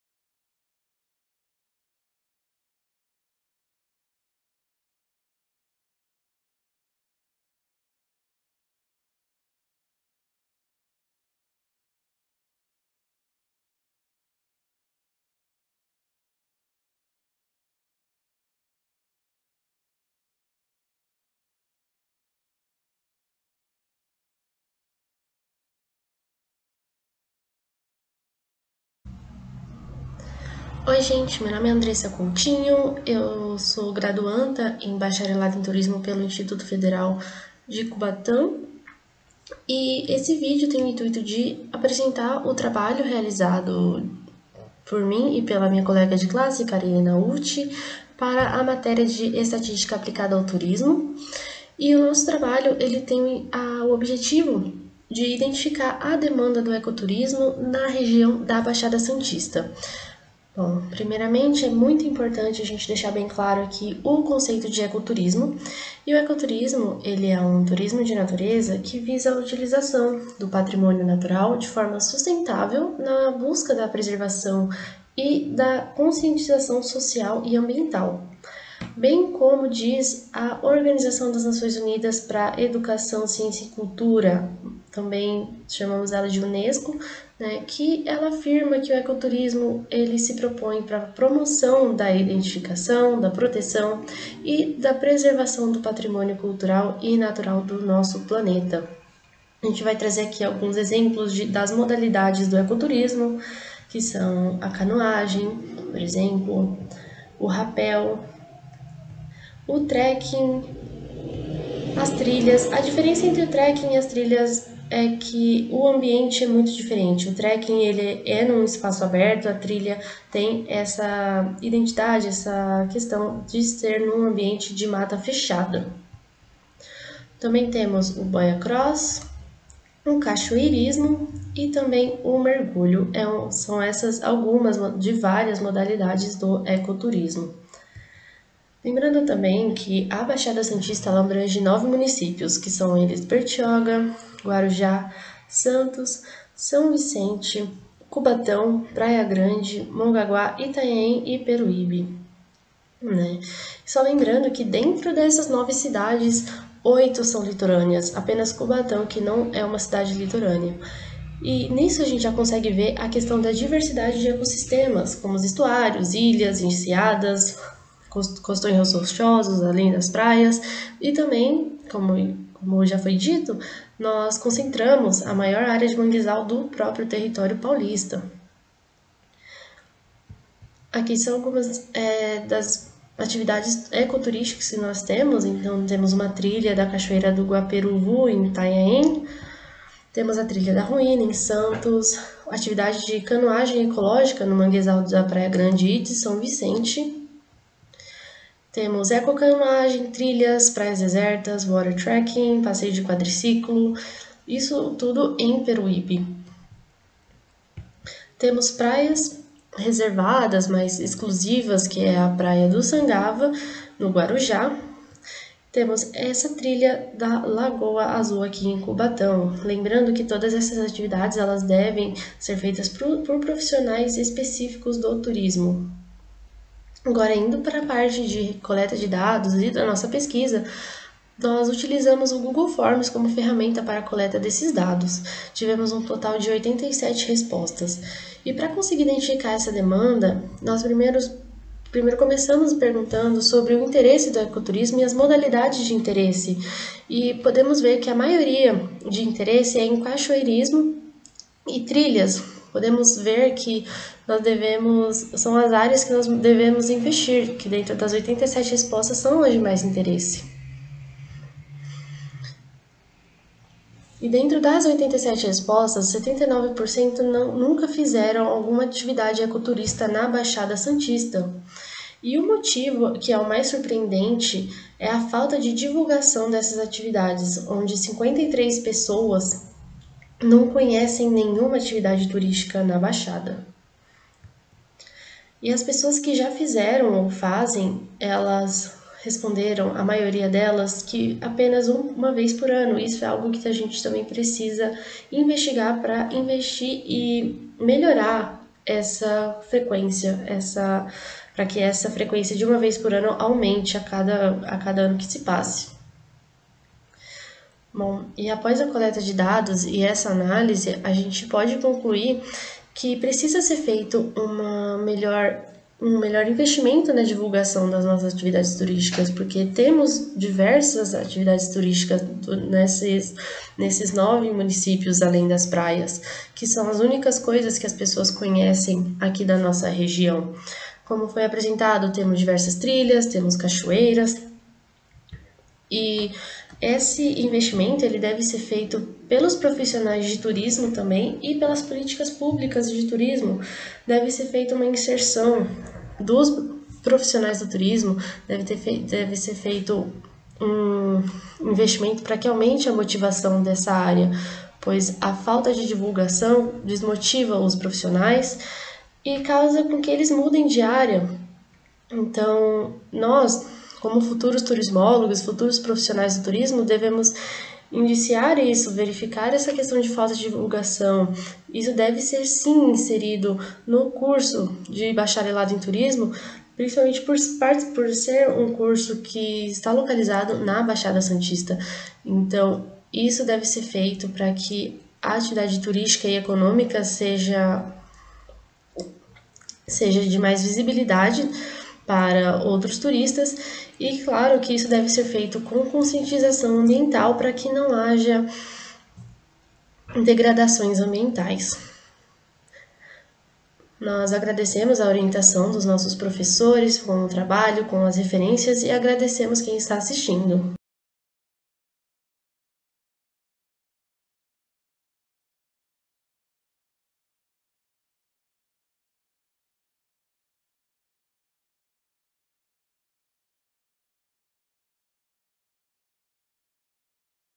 Demanda do Ecoturismo na baixada santista
Vídeo elaborado pelos estudantes do curso Bacharelado em Turismo do IFSP Cubatão, terceiro semestre, como parte da estratégia didática do professor para a disciplina - Estatística aplicada ao Turismo ESTT3
discentes: Andressa Coutinho de Faria, Carinna Ucci de Menezes
Docente: Maria Jeanna Sousa dos Santos Oliveira